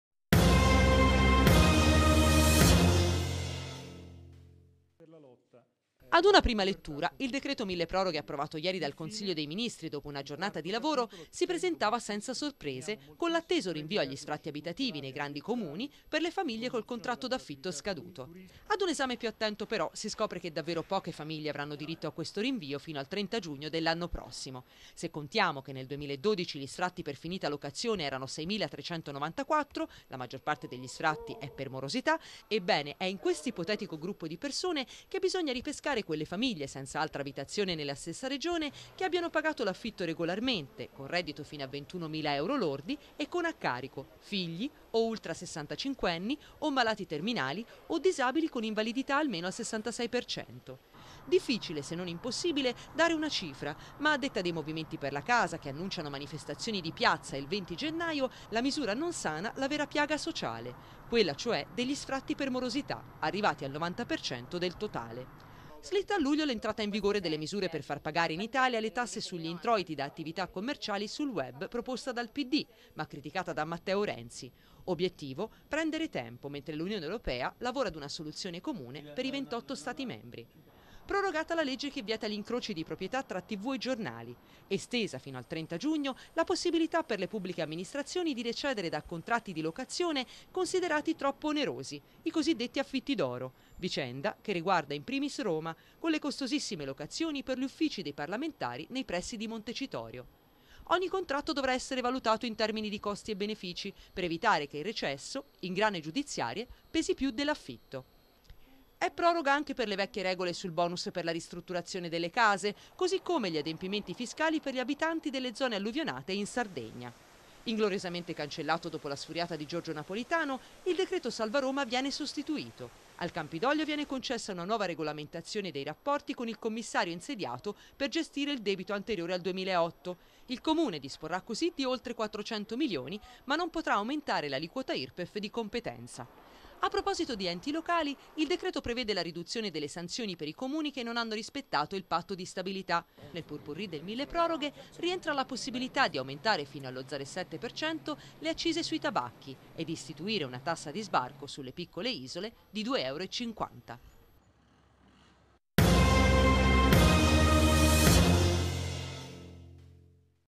Milleproroghe, movimenti per la casa contro il decreto
Il rinvio degli sfratti fino al giugno del 2014 non risolve a loro giudizio il vero problema: gli sfratti per morosità. Servizio di Sonia Mancini